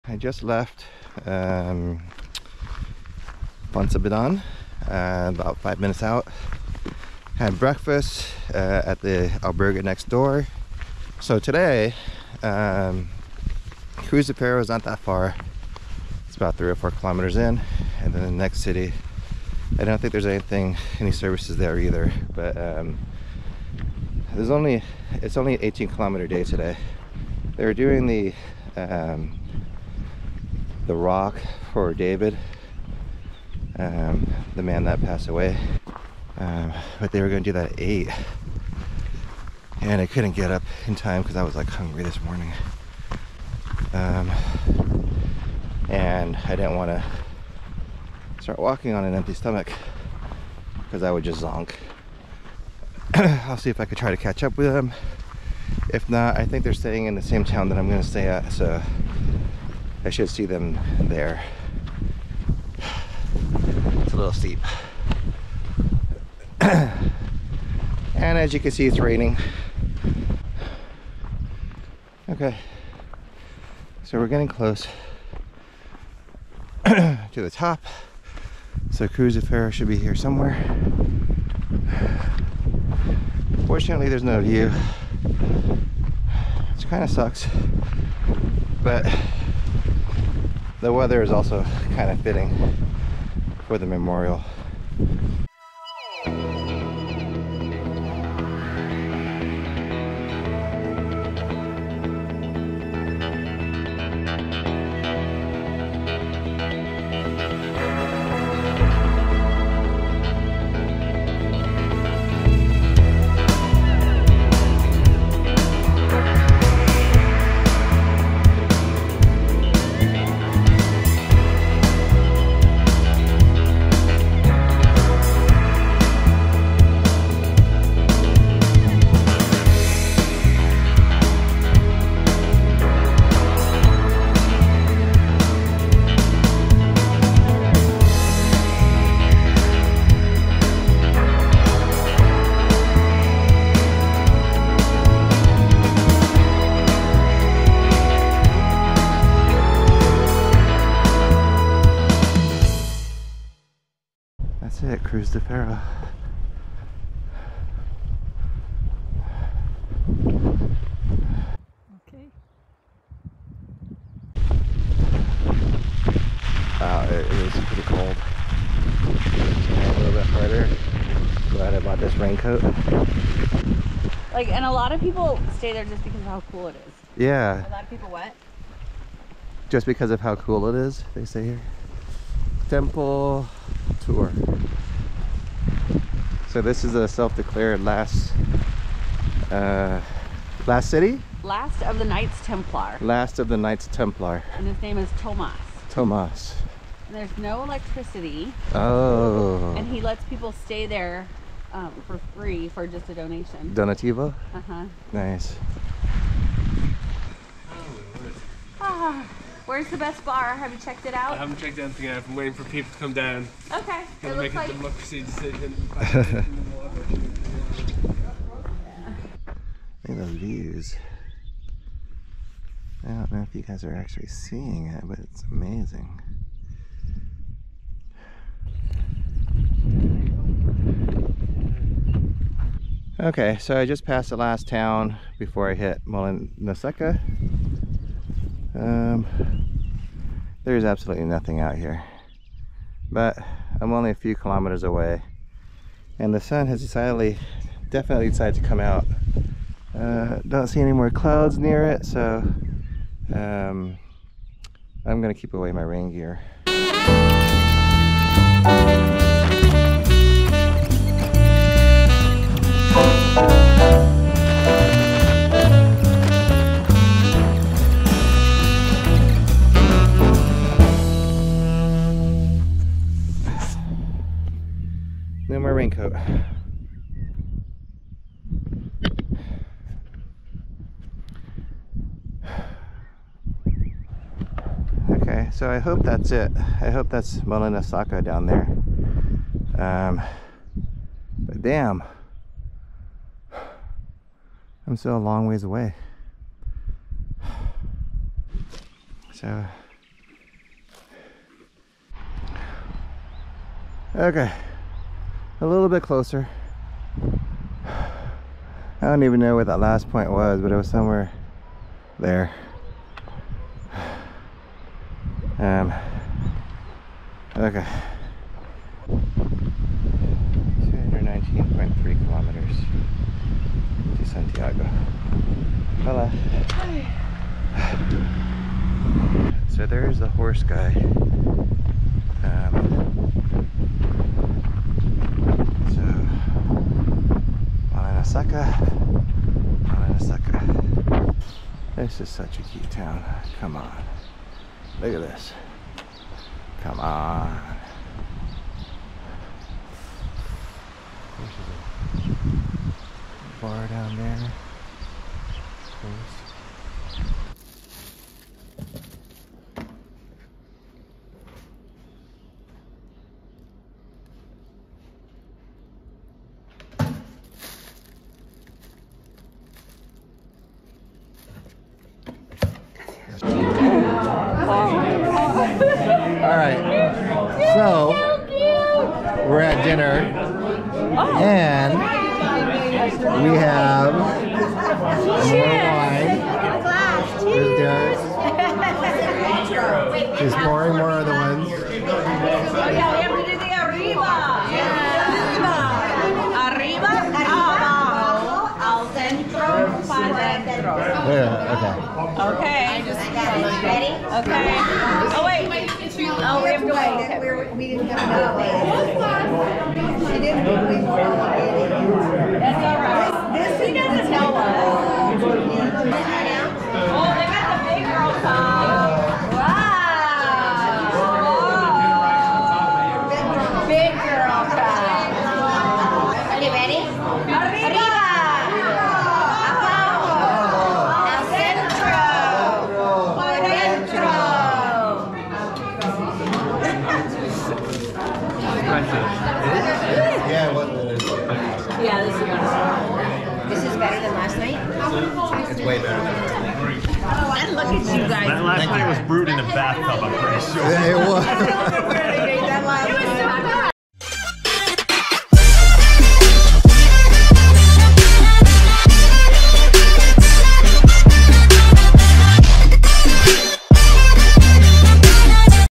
Vlog 29 - The Cruz de Ferro | Foncebadón to Molinaseca | Camino de Santiago - Frances
Vlog 29 - The Cruz de Ferro

Day 29 of my Camino Frances. Foncebadón to Molinaseca. April - May 2023